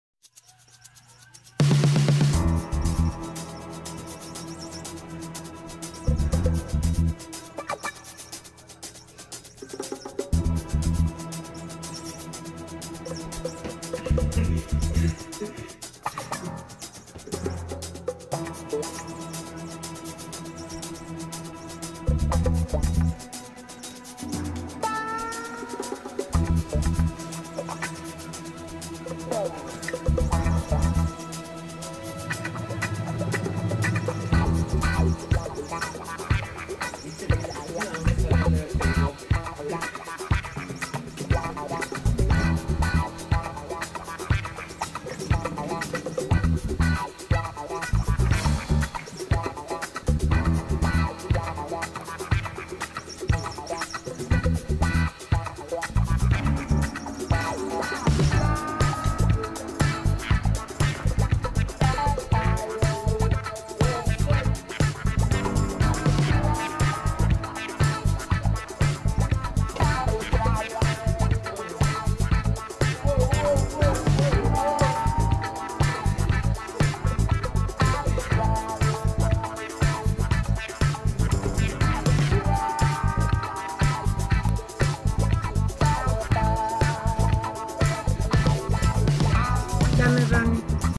Miniature Pinscher VS a Rat
Brownie battled to keep her bed safe.